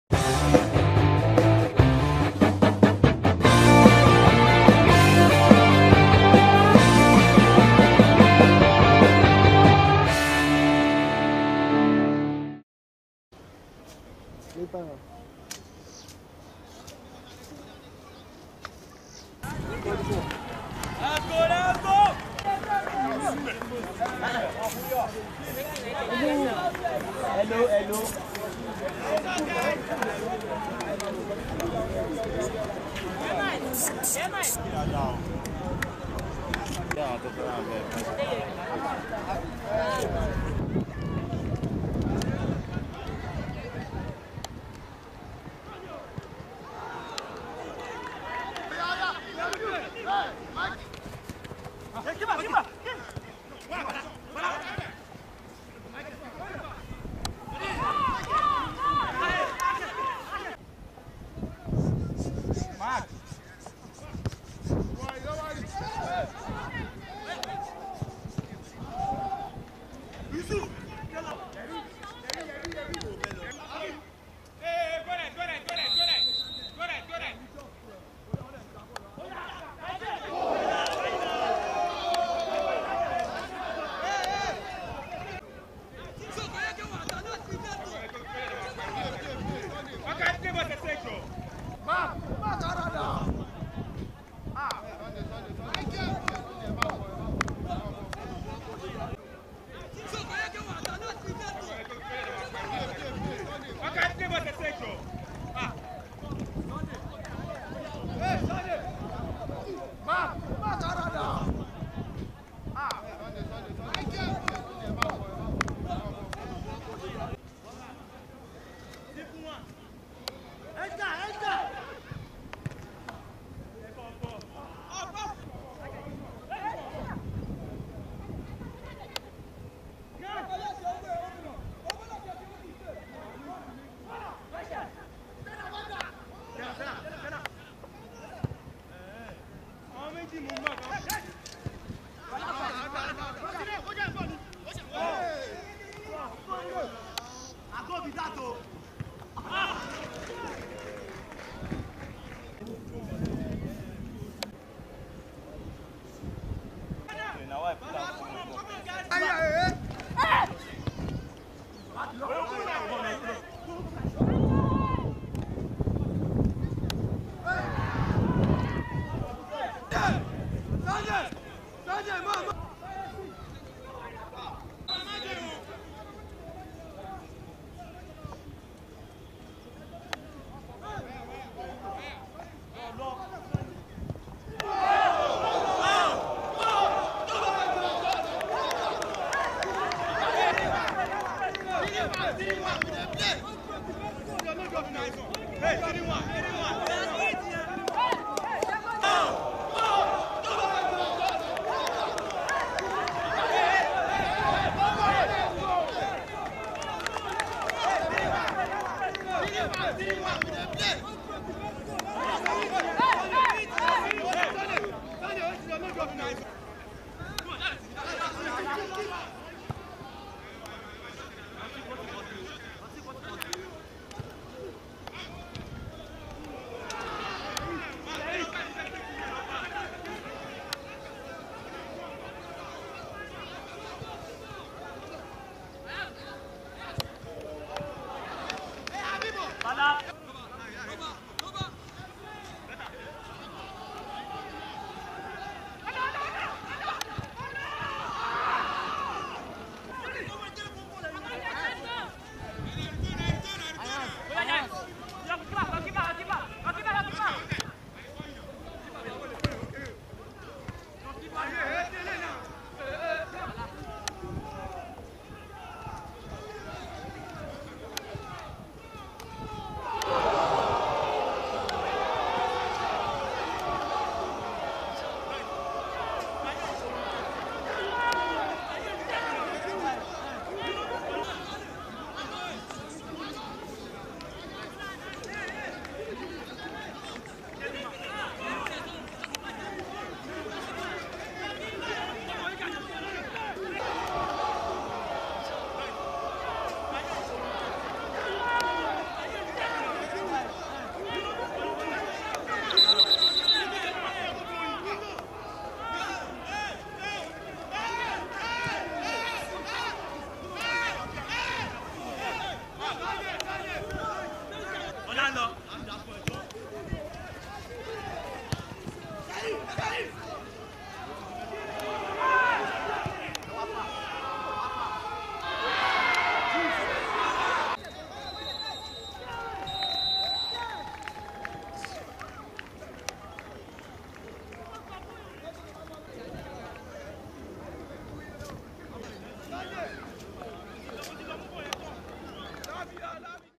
Lekki Utd vs Ablaze FA Matchday 1 Exclusive Highlights NLO 2023 Season
Lekki Utd 2 : 1 Ablaze FA...